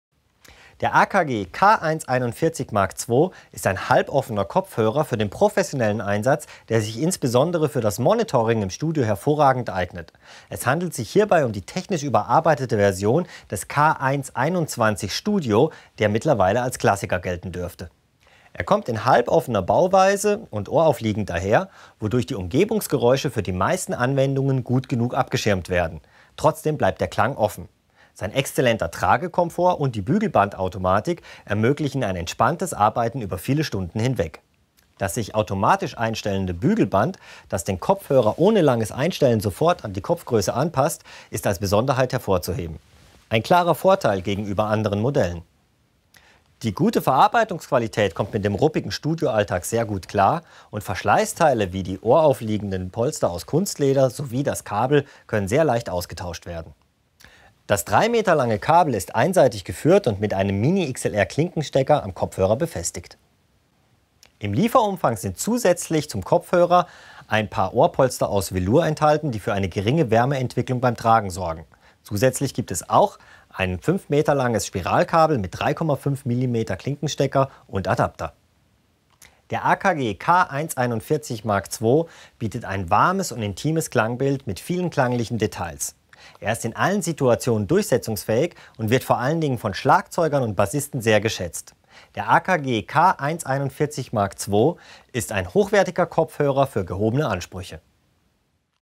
AKG K141 MkII
Der AKG K141 MkII ist ein halboffener, ohraufliegender Studiokopfhörer. Leder- und Velours-Ohrpolster sowie Bügelband-Automatik bieten hohen Tragekomfort. Die niedrige Impedanz ermöglicht auch den Einsatz an Computern und Geräten mit geringer Ausgangsspannung.